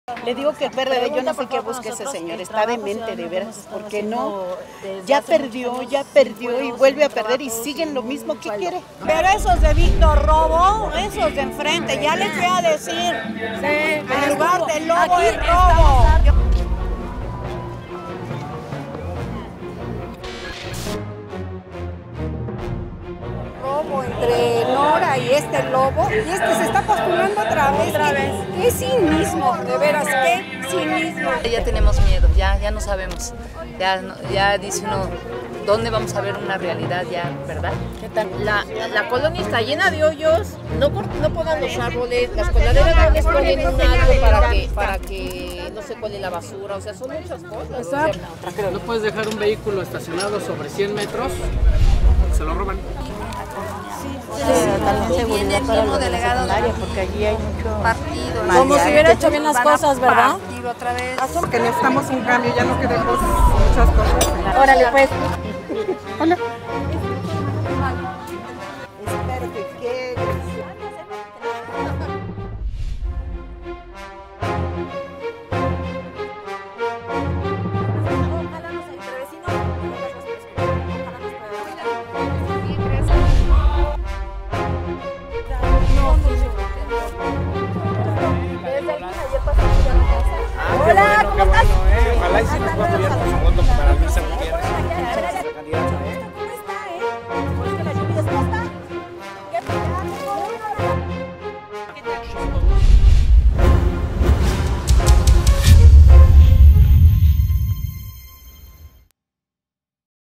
Recorrido en GAM
14 de mayo 2015

La candidata del PAN  al Distrito 2 local de GAM Luisa Adriana Gutiérrez Ureña  en compañía de Mauricio Tabe Echartea presidente Regional, realizaron un recorrido por el tianguis que se ubica alrededor del deportivo Miguel Alemán  y donde  le expresaron que les ayude a detener la corrupción de las autoridades delegacionales y la inseguridad que priva en la zona, así como,  intervenga para que  las vialidades de la colonia no sean afectadas por la línea 6 del metrobús.